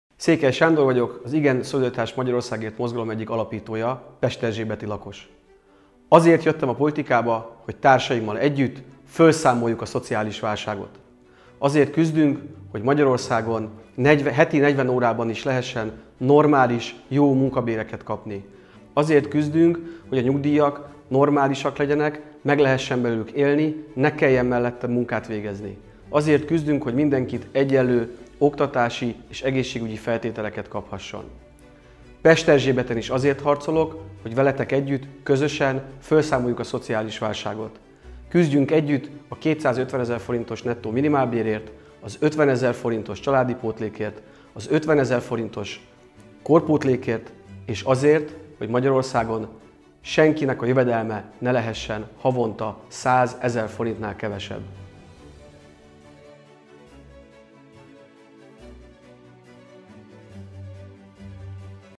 Székely Sándor – Budapest 16. számú egyéni választókerület – ISZOMM 2022
Székely Sándor vagyok országgyűlési képviselő, az Igen Szolidaritás egyik alapítója, pesterzsébeti lakos.

Azért jöttem a politikába, hogy társaimmal együtt fölszámoljuk a szociális válságot.

Azért küzdünk, hogy Magyarországon heti 40 órában is lehessen normális és jó munkabéreket kapni.

Azért küzdünk, hogy a nyugdíjak normálisak legyenek, meg lehessen belőlük élni, ne kelljen mellette munkát végezni.

Azért küzdünk, hogy mindenki egyenlő oktatási és egészségügyi lehetőséget kaphasson.

Pesterzsébeten is azért harcolok, hogy veletek együtt közösen fölszámoljuk a szociális válságot.

Küzdjünk együtt a 250 ezres nettó minimálbérért, az 50 ezer forintos családi pótlékért, 50 ezer forintos korpótlékért és azért, hogy Magyarországon senkinek a jövedelme ne lehessen havonta 100 ezer forintnál kevesebb.

IGEN SZOLIDARITÁS MAGYARORSZÁGÉRT MOZGALOM